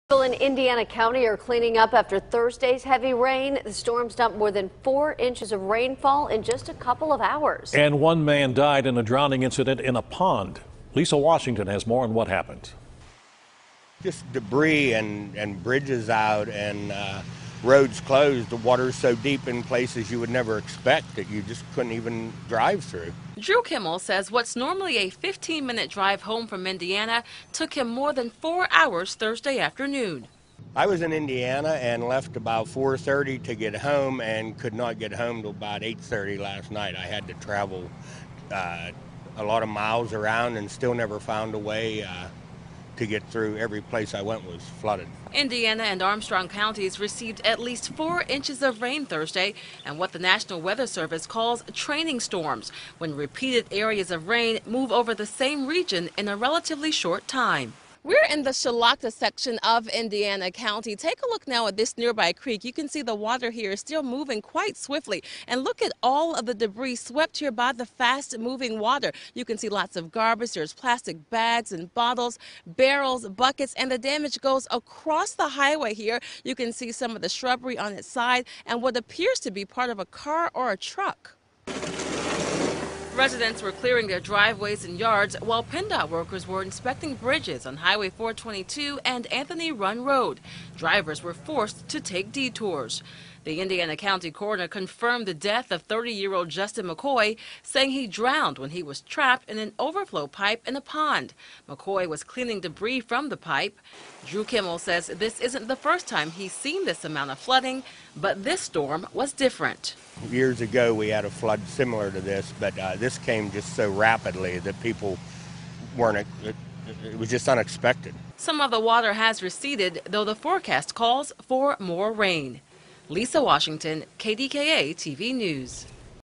Indiana County Residents Cleaning Up After Latest Round Of Flooding
People in Indiana County are cleaning up after Thursday's heavy rain.